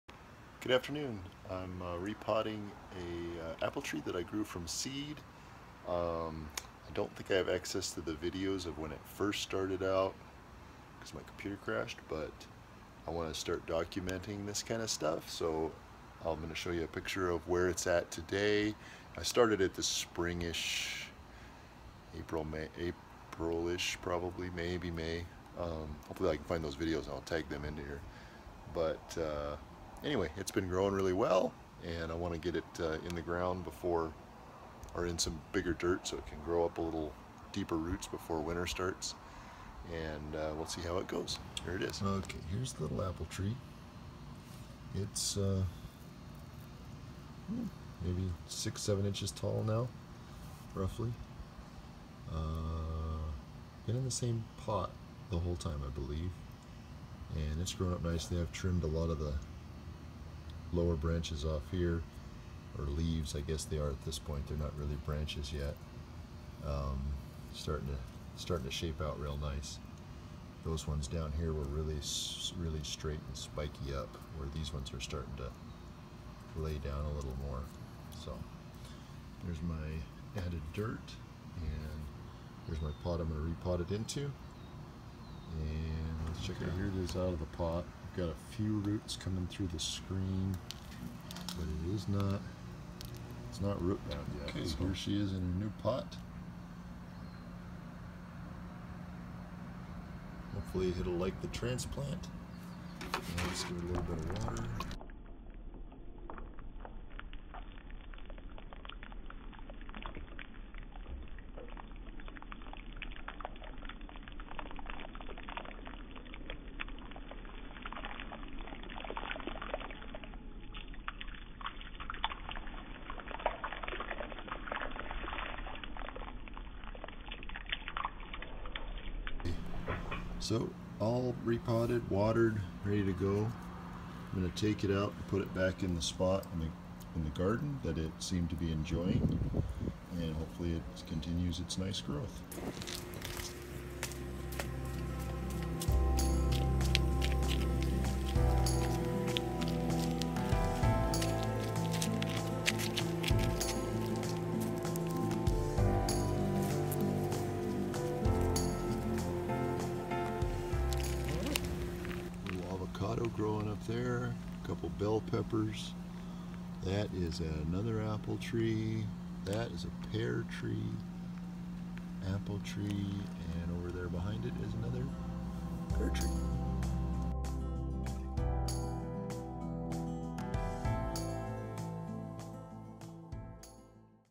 Apple tree from seed update 8.18.17
Repotting an apple tree that I started from seed